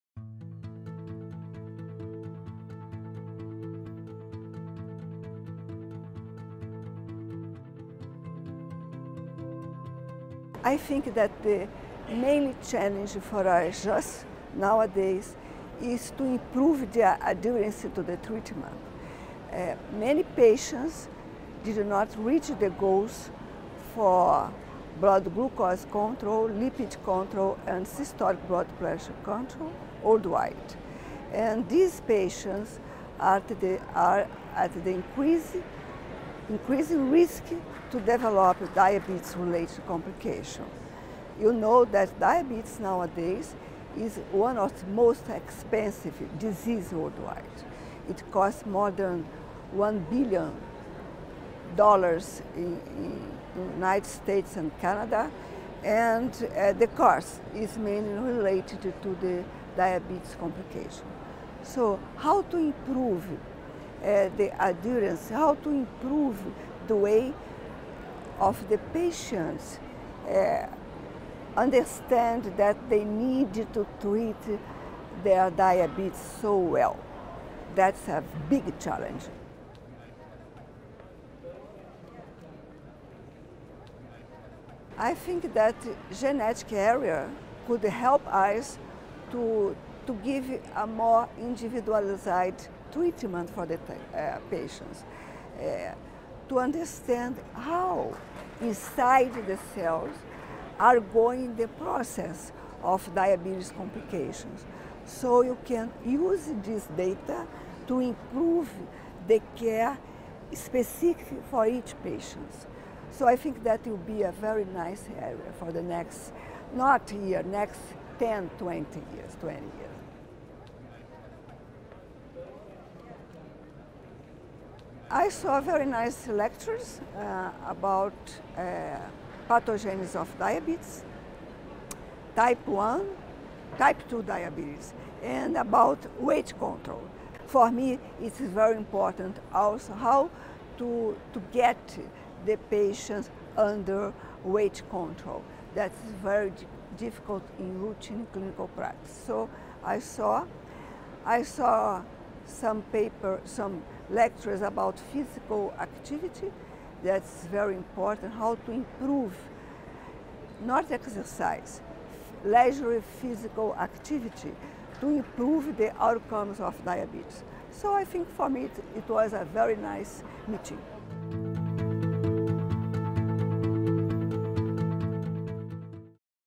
EASD 2017 - Marilia Gomes Interview
Marilia Gomes discusses her highlights of EASD and the challenges she faces treating patients with type 2 diabetes.